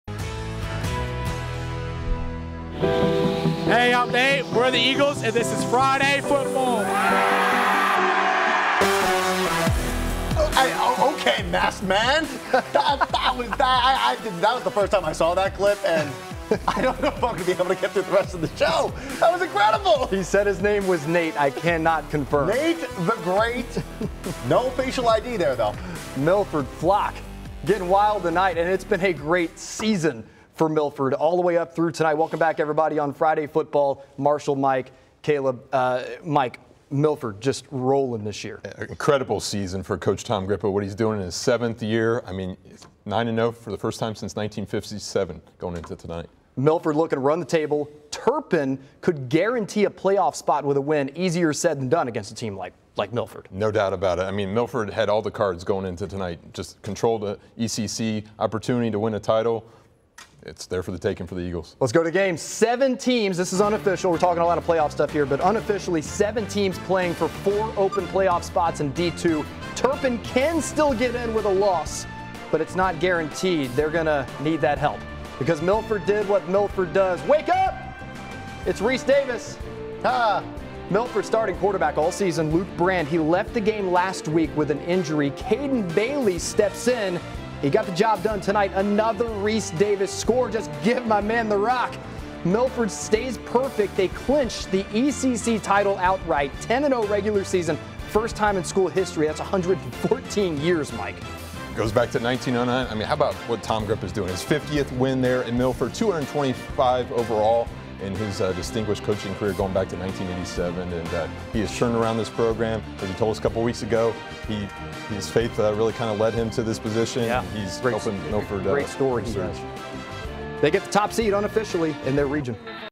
Milford ends their season undefeated with win over Turpin
For the first time in the school's 114-year history, Milford went undefeated with a 10-0 regular season. Their final regular season victory against Turpin was more of what they've done all year - score. The 42-21 convincing victory awards Milford the outright ECC Title and top seed in the D1, Southwest region. Turpin could still get into the playoffs, but they'll have to wait for the final rankings to confirm.